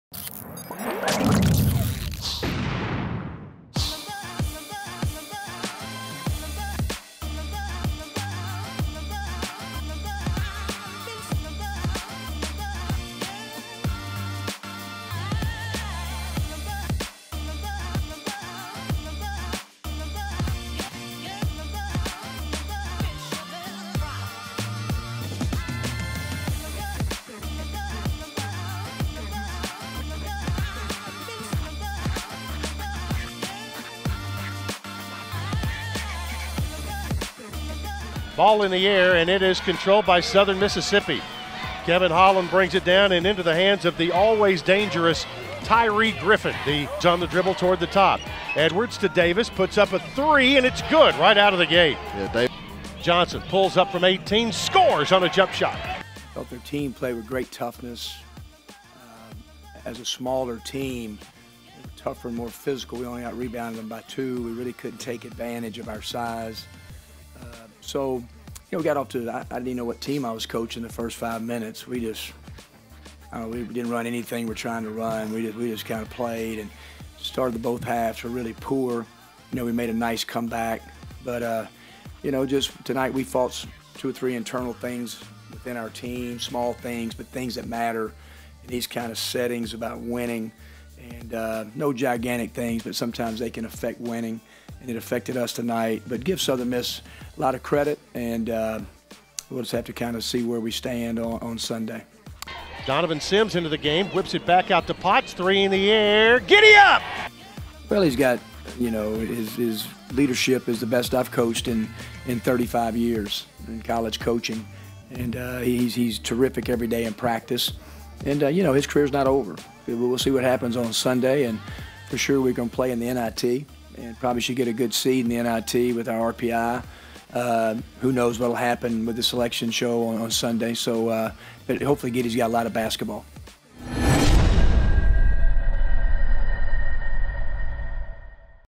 vs USM - CUSA Quarterfinals Highlight
Blue Raiders fall 71-68 to Southern Miss in the Quarterfinals of the Conference USA Tournament.